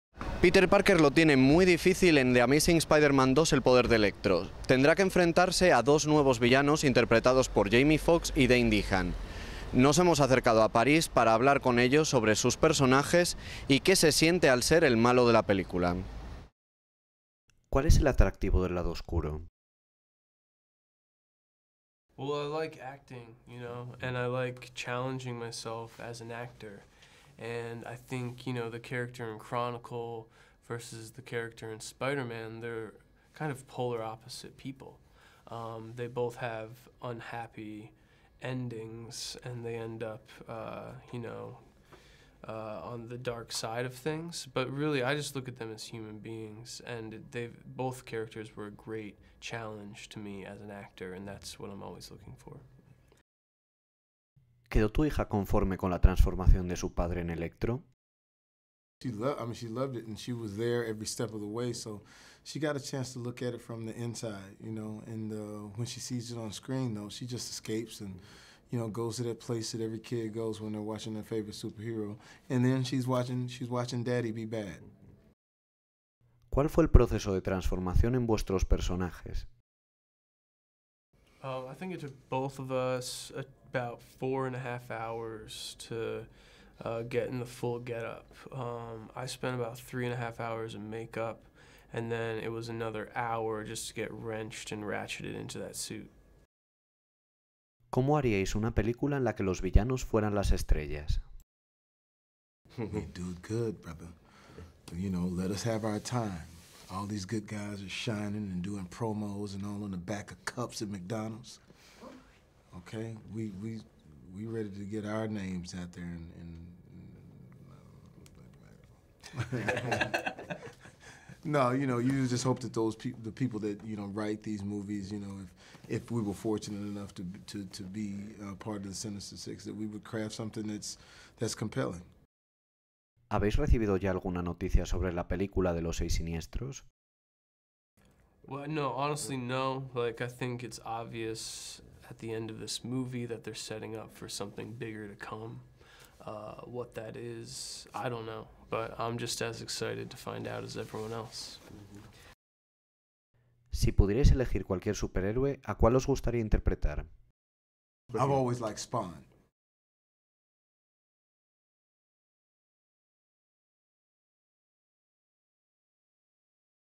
'The Amazing Spider-Man 2: El poder de Electro': Entrevista a Jamie Foxx y Dane DeHaan
Hablamos con los dos villanos de la película de Marc Webb. Jamie Foxx interpreta a Electro, mientras que Dane DeHaan se convierte en Harry Osborn. En cines el 17 de abril.